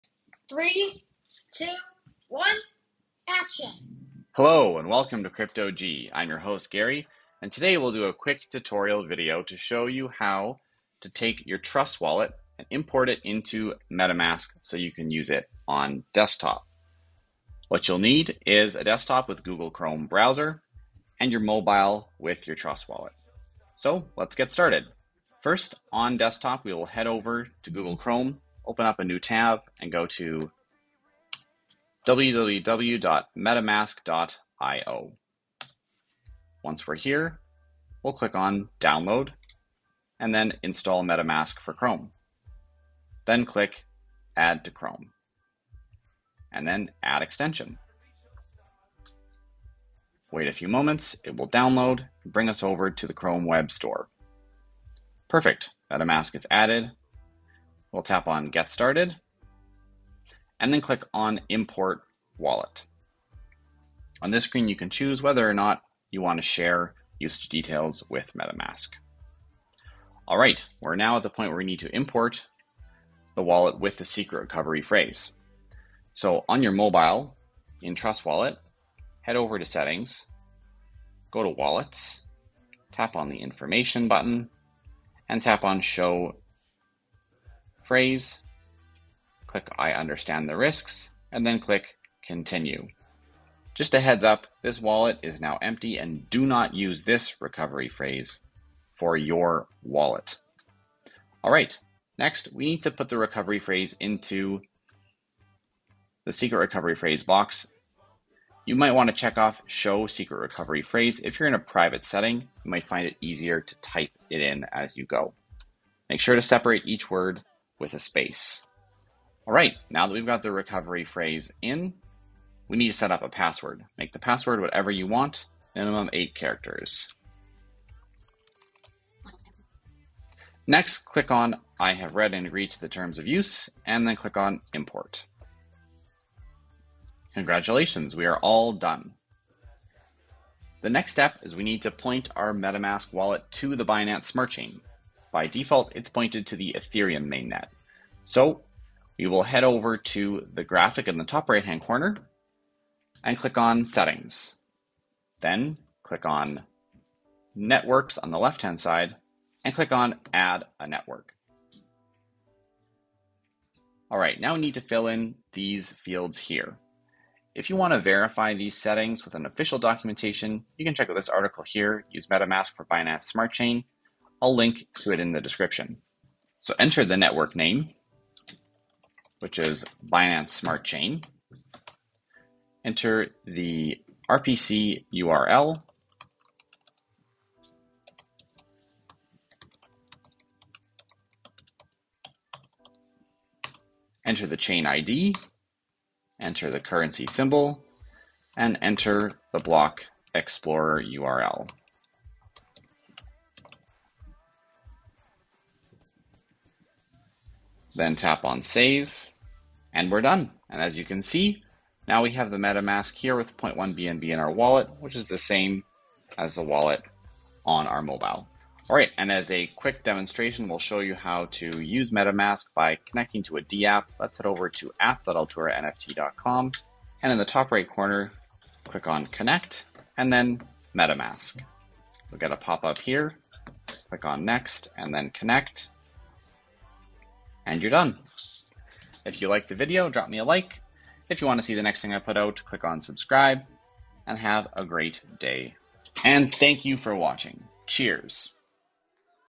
How to load Trust Wallet into Metamask and point to Binance Smart Chain
This video will show you how to use your existing Trust Wallet that you have on your mobile device and bring it over to Metamask in Google Chrome browser on desktop and then point to the Binance Smart Chain so you can use BSC dApps connected to your wallet.

🎶 Do you want to be rich? 😉